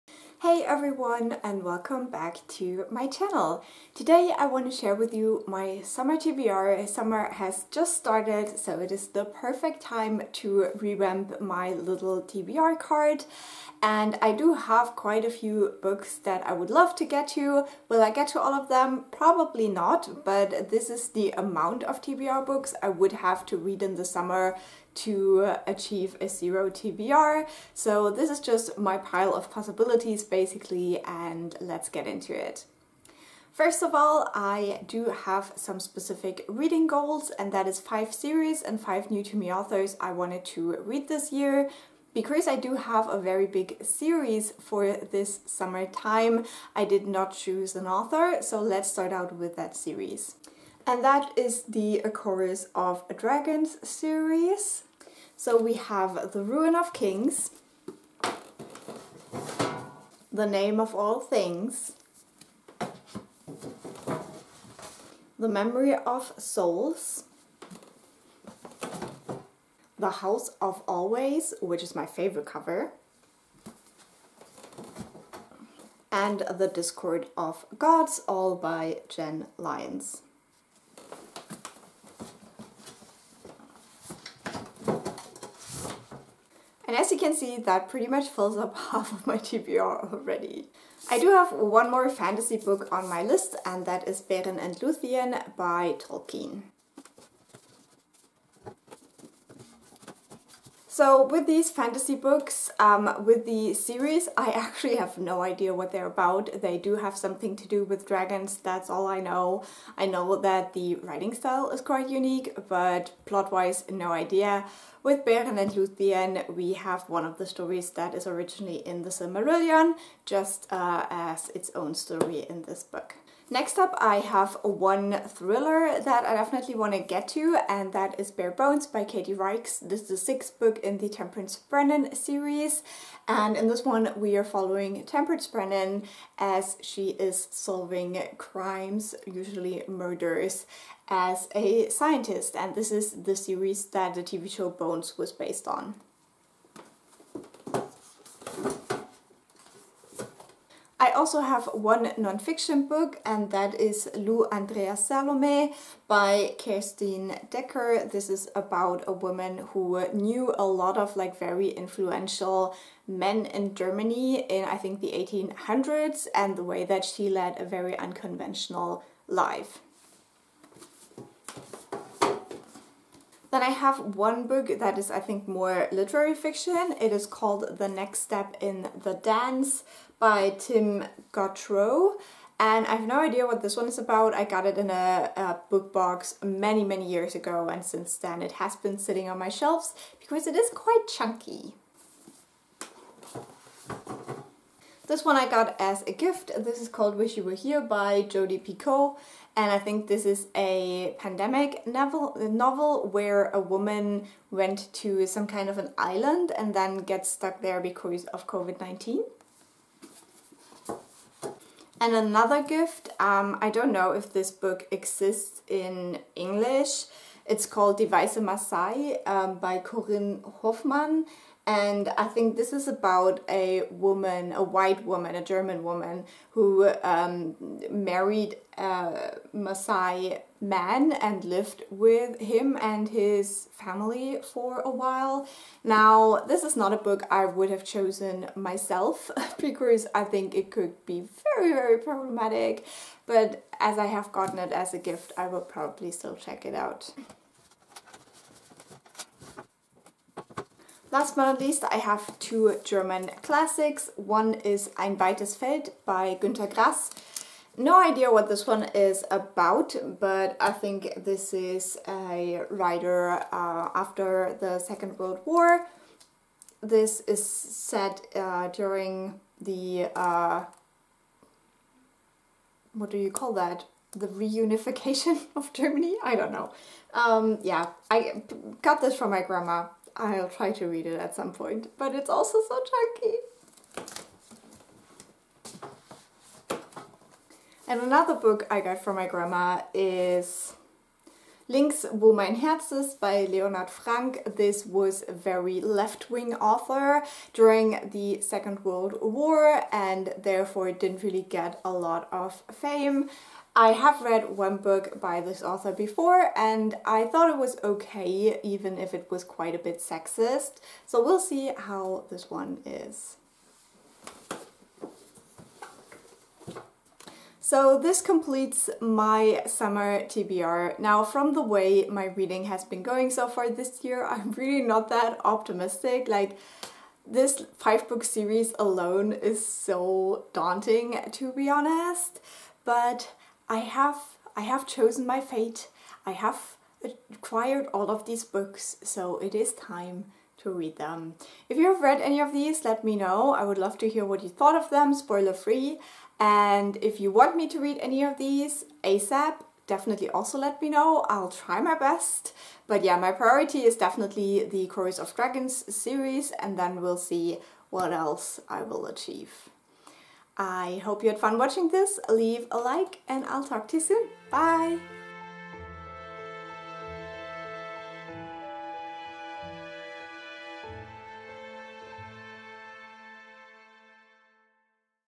My Summer TBR 2024 | The Books I wanna read this Summer
As always, this is more a pile of possibilities, and we will see how much I can actually achieve. Anything you want me to prioritize?

My recent wishlist